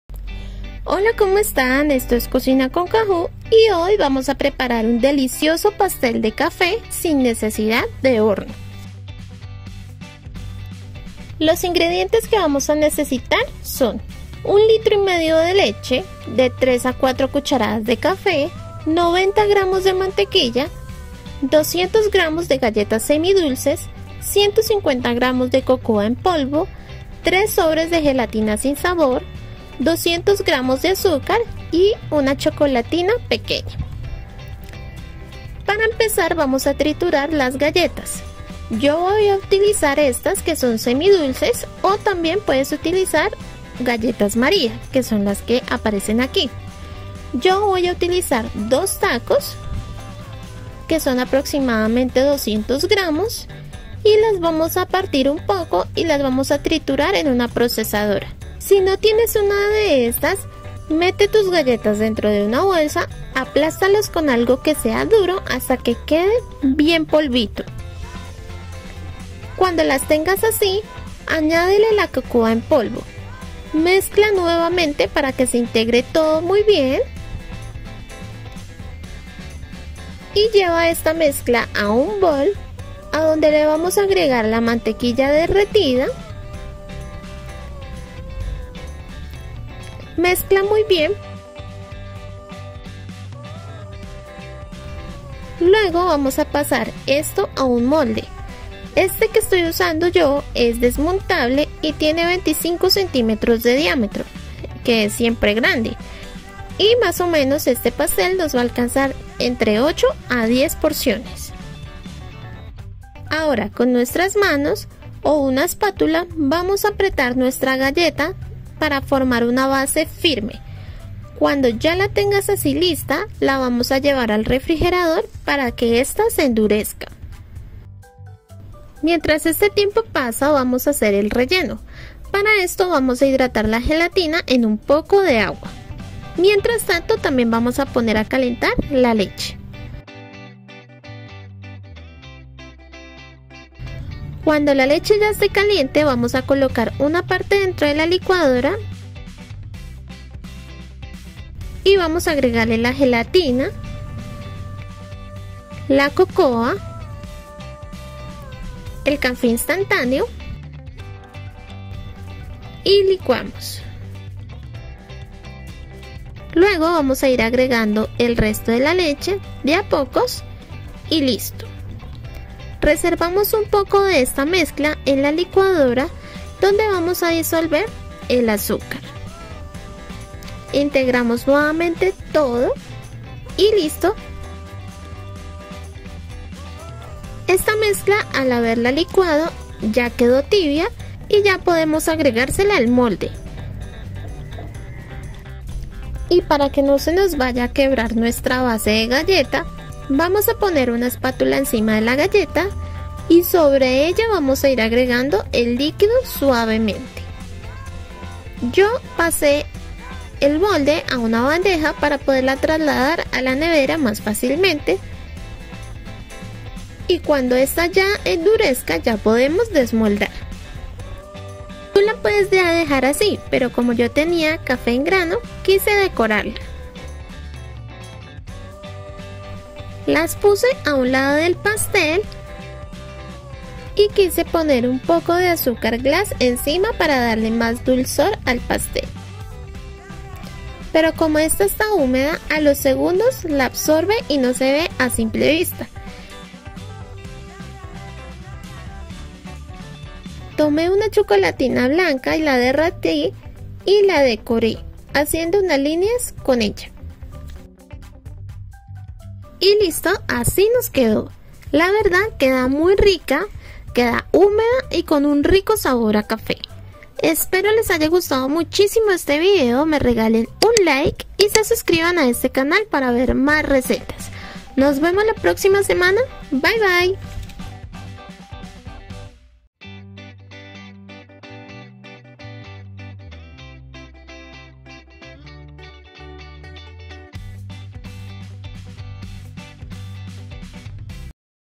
Pastel Frio de Cafe/ SIN HORNO/Receta Facil
Hola como están? Espero que muy bien, en el video de hoy vamos a preparar una pastel frio de café, el cual no necesita horno y no requieres de muchos ingredientes, es fácil de preparar y te sorprenderá su sabor. Espero les guste mucho y me apoyen con un like y compartiendo este video